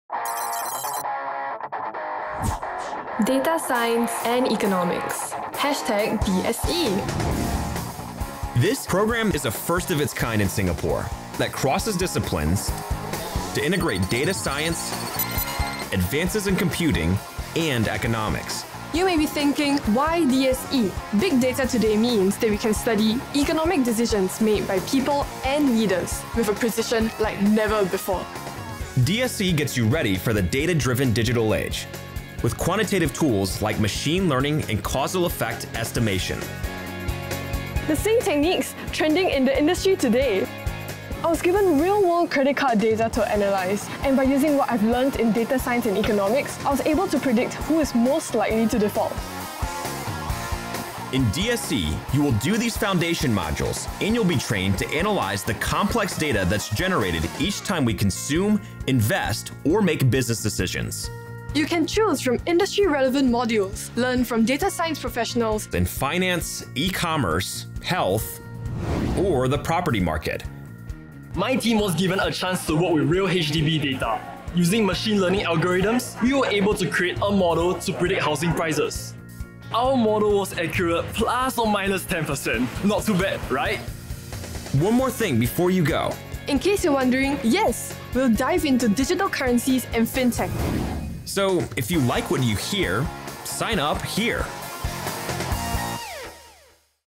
Data Science and Economics | XDP@CHS Major
Big Data has created opportunities to study economic systems and choices with a higher degree of precision using data science. This programme tightly integrates knowledge from the complementary disciplines of data science and economics to address business and societal needs arising from the digitalisation of the economy and banking liberalisation.